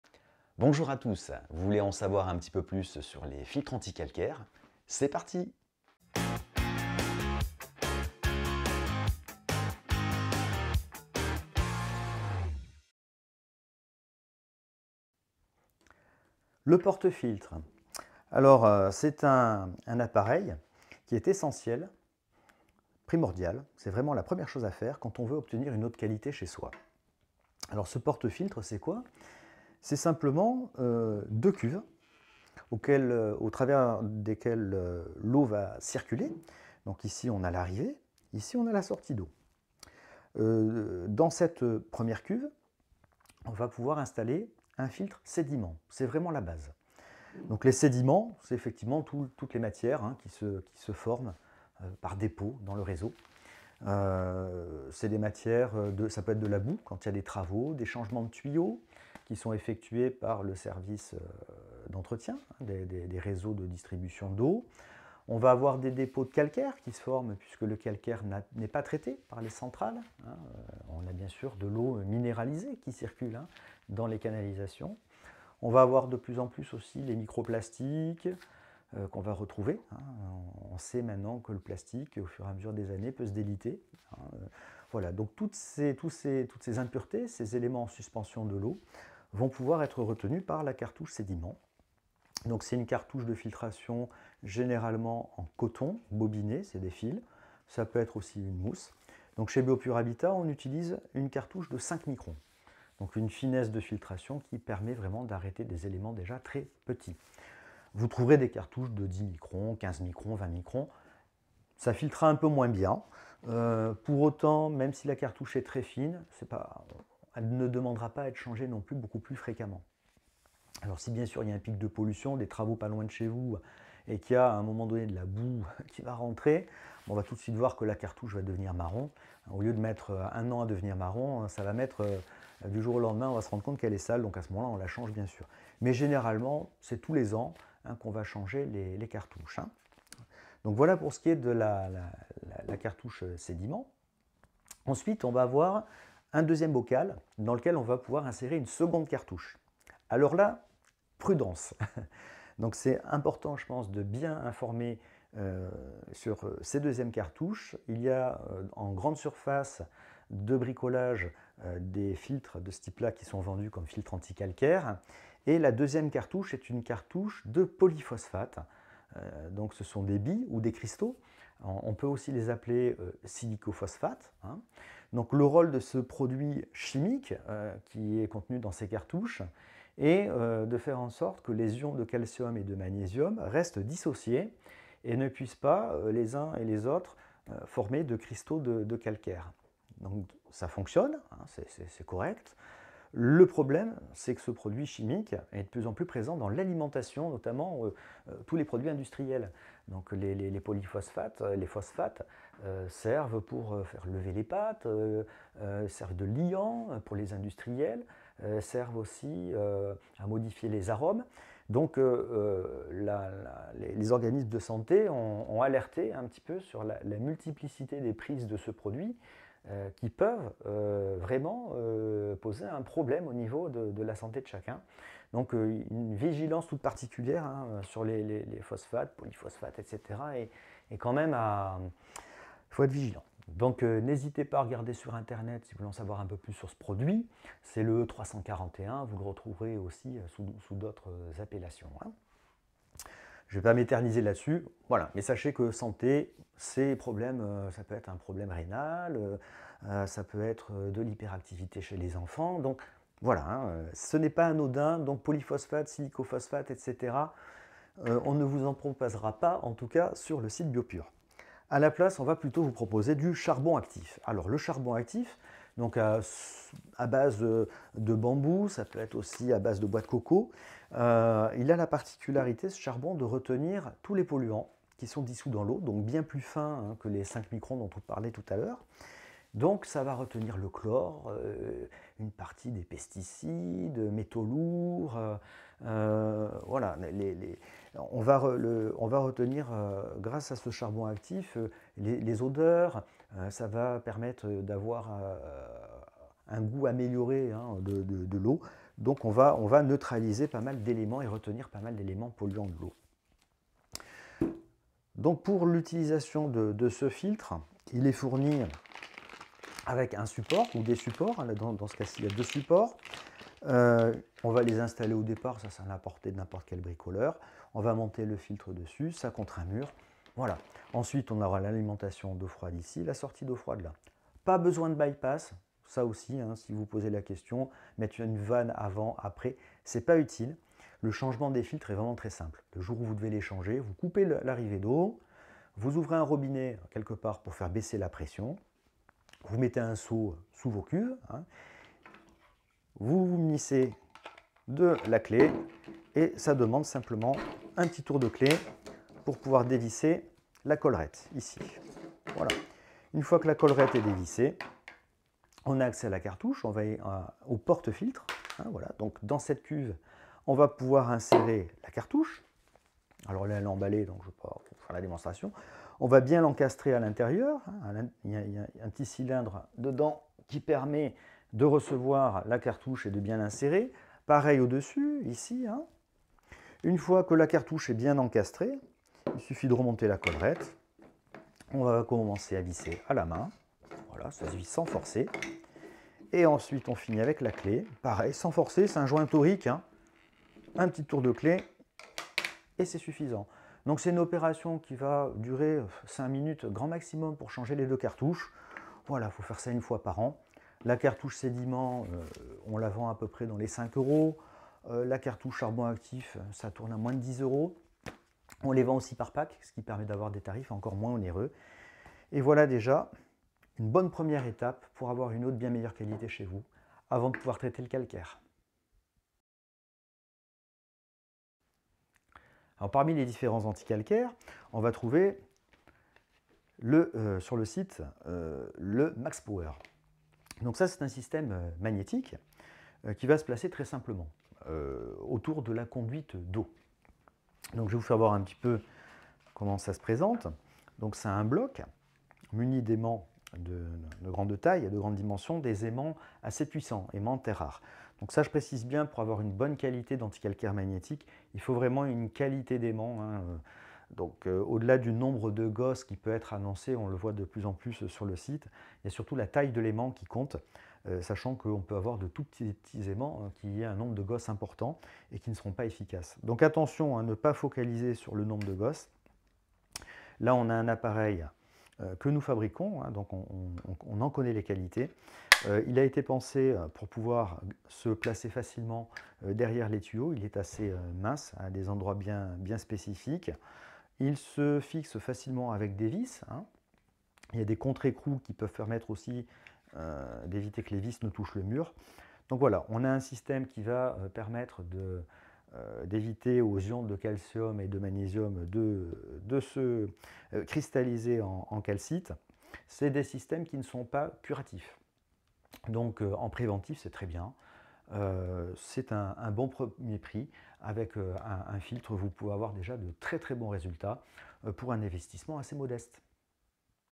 Filtre anti calcaire magnétique 12800
Un filtre anti calcaire efficace et écologique.
Quelques explications sur le fonctionnement de ce type de filtre anti calcaire, financièrement abordable, simple et efficace.

Vous pourrez trouver plus de détails technique sur notre site et sur la page produit